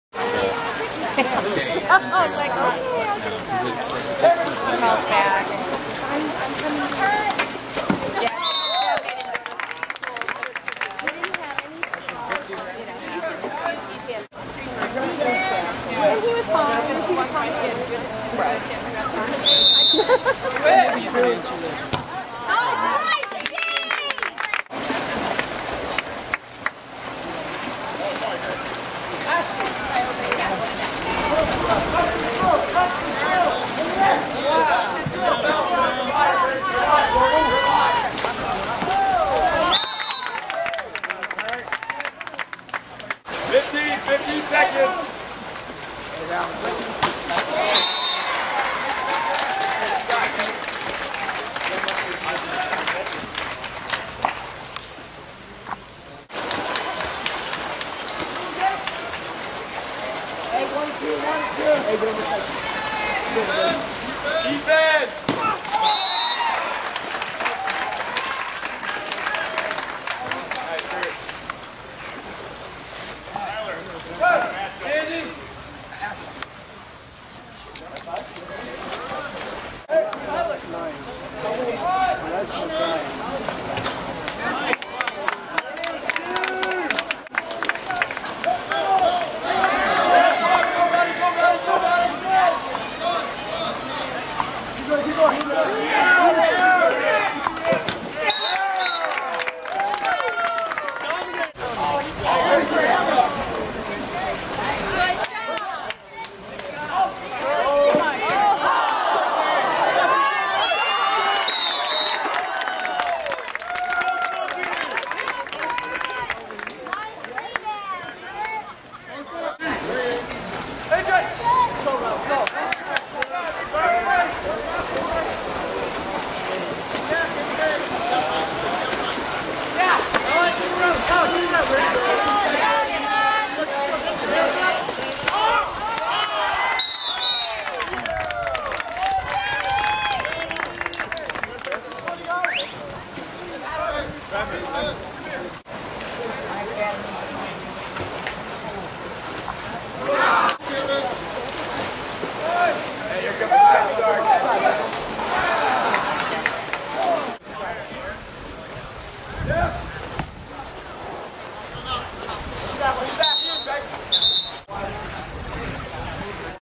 Chadwick School vs. Redondo Union High -- Water polo highlights 9-24-09
Highlights from Chadwick School's 10-0 victory over Redondo Union High on 9-24-09 at Chadwick.